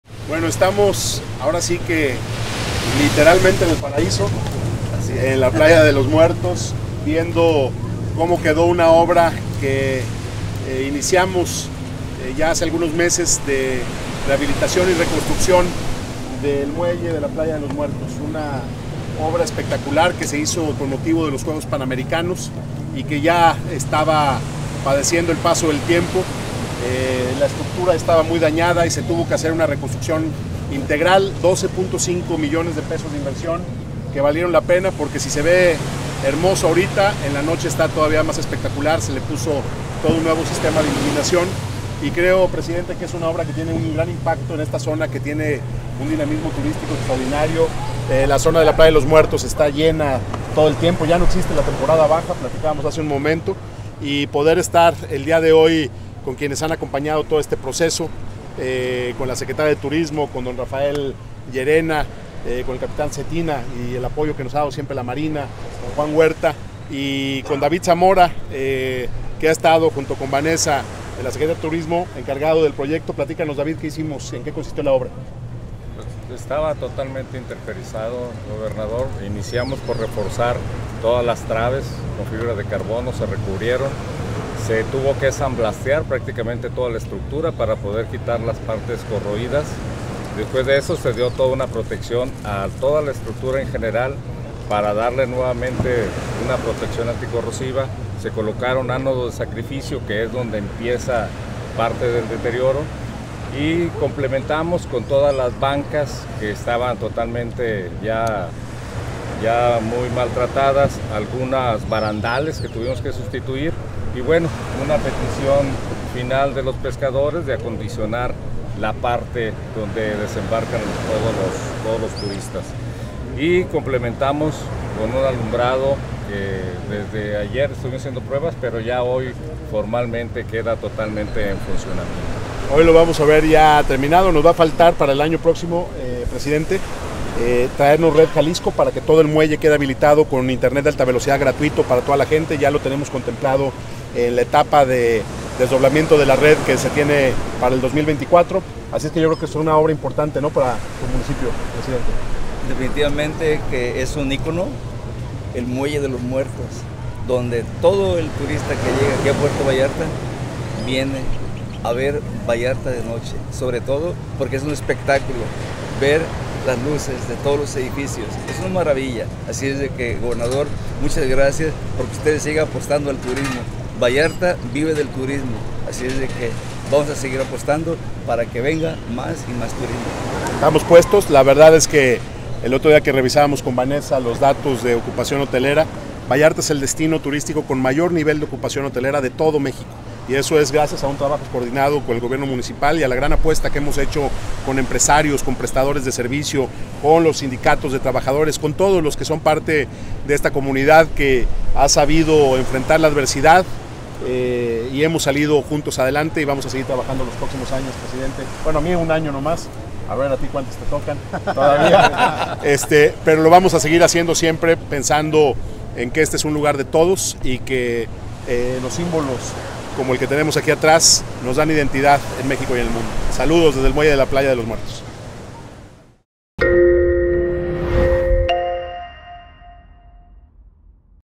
Puerto Vallarta | Inauguración del muelle de playa Los Muertos.
Aprovechamos la visita a PuertoVallarta para inaugurar el muelle de playa Los Muertos, un ícono de la ciudad que ya estaba padeciendo el paso del tiempo y que, con 12.5 mdp y un gran trabajo de renovación, recuperó toda su grandeza. Aquí te contamos los detalles